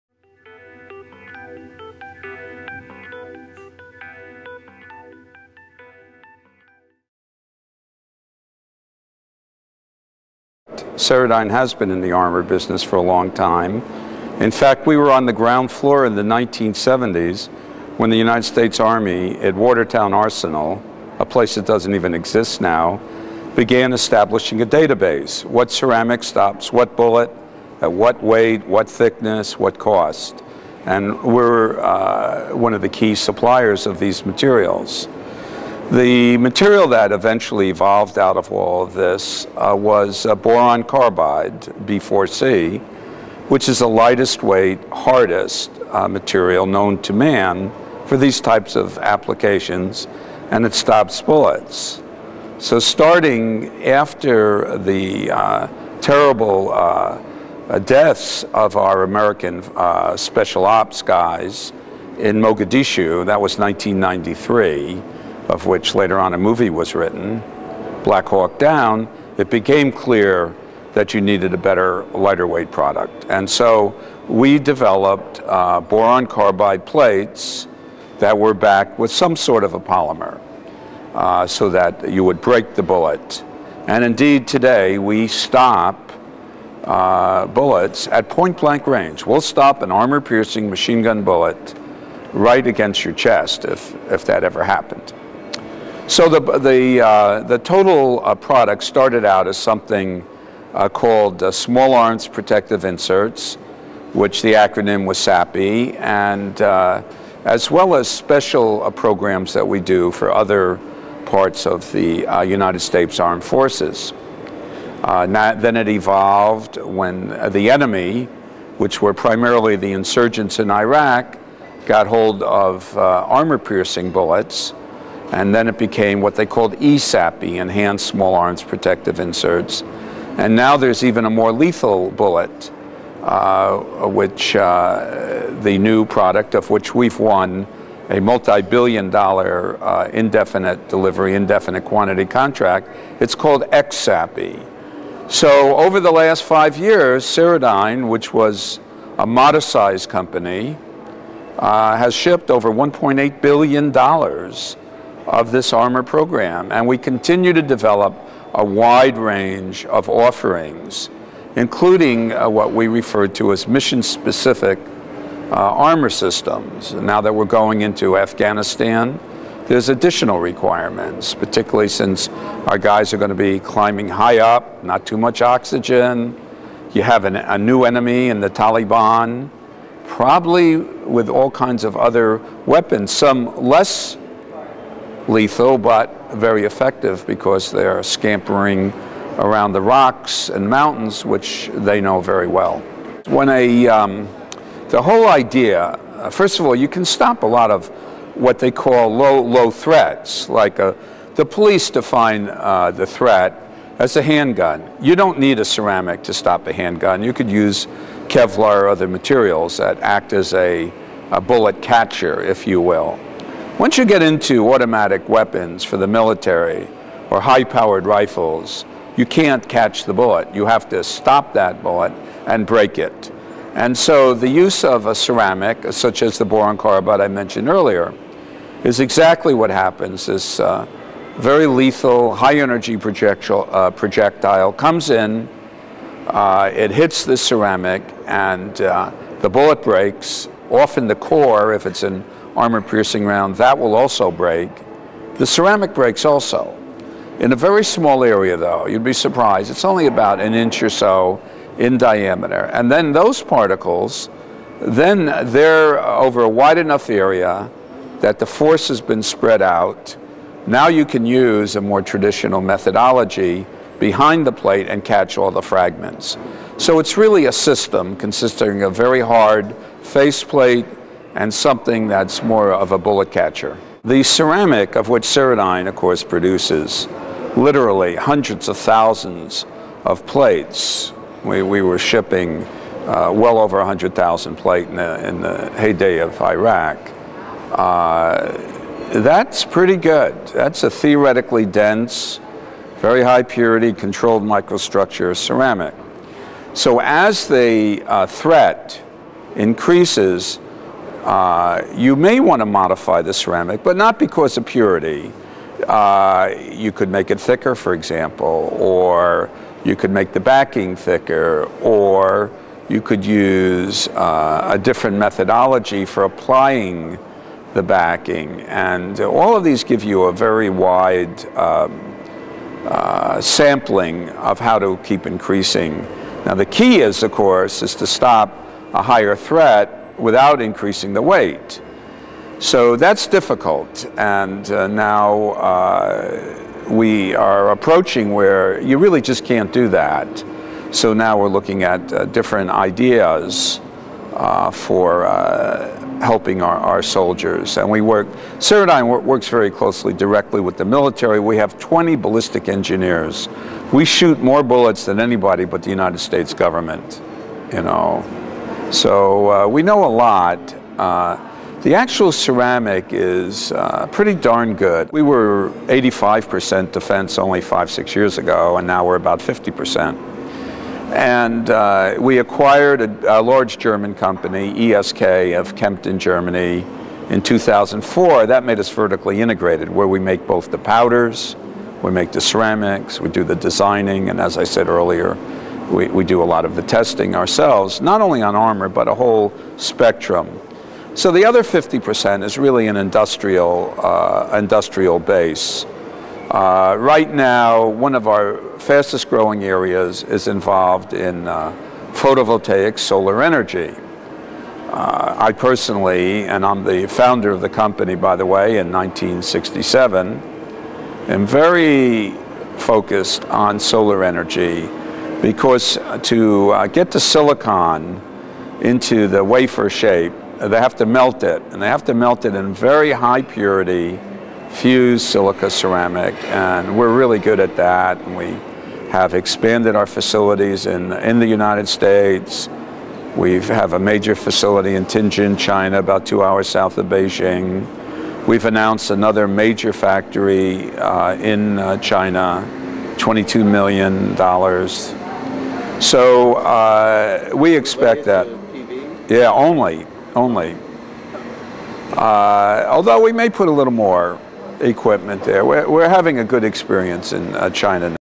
Ceradyne's Joel Moskowitz, pt. 1
Here is a chance to get, first-hand, a business report from one of the United States most successful ceramic products company. Ceradyne Corporations founder and CEO, Joel Moskowitz, describes the history and ceramic technology behind the ceramic armor products his company is best known for. But, interestingly, he also describes some the the companys strategy to expand into non-armor areas such as specialty crucibles for preparing solar-grade polysilicon, cosmetics raw materials and ceramic replacements for carbon cathodes used in smelting aluminum. He says Ceradyne should be known as an advance technical ceramic company and not just a defense company. Next year they may call us a solar company and in five years maybe they will call us an aluminum company. He talks about the current economic environment and we learn how, even in a recession, Moskowitz has kept Ceradyne cash-rich and in a good position to make acquistions for the Ceradyne portfolio at discount prices.